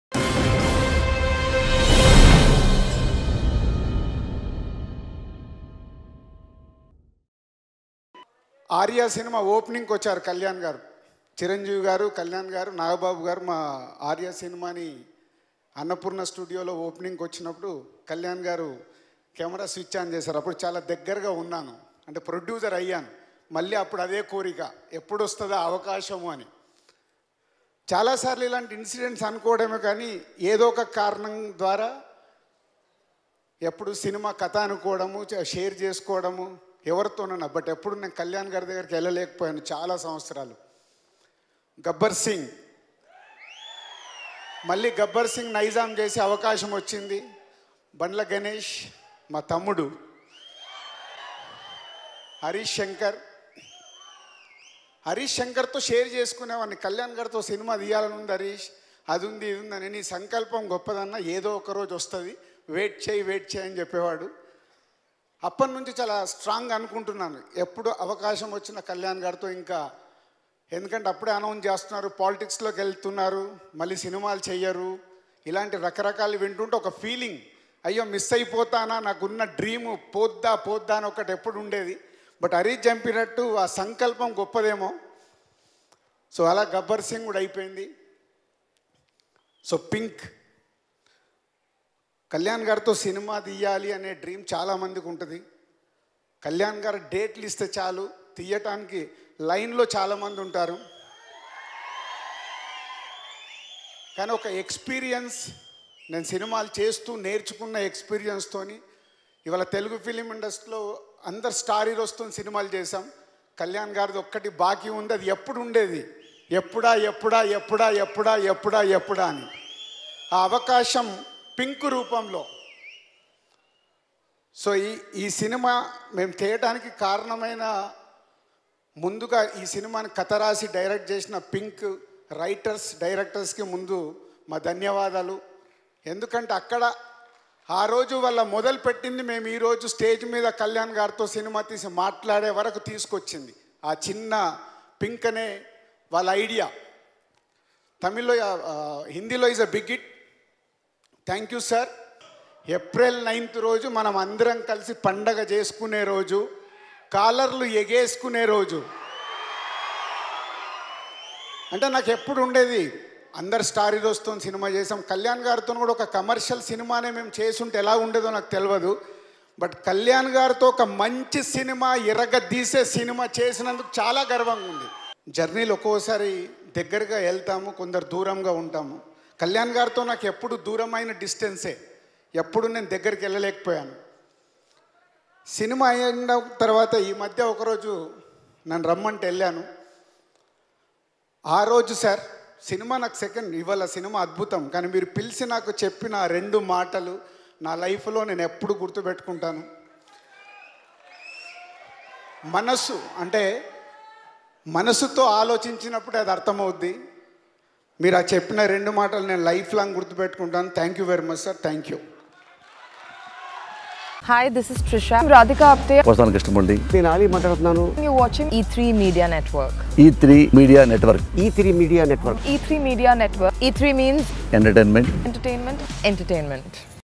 Dil Raju Mass Speech At Vakeel Saab Pre Release Event | Pawan Kalyan | E3 Talkies
Watch Here: Dil Raju Mass Speech At Vakeel Saab Pre Release Event | Pawan Kalyan | E3 Talkies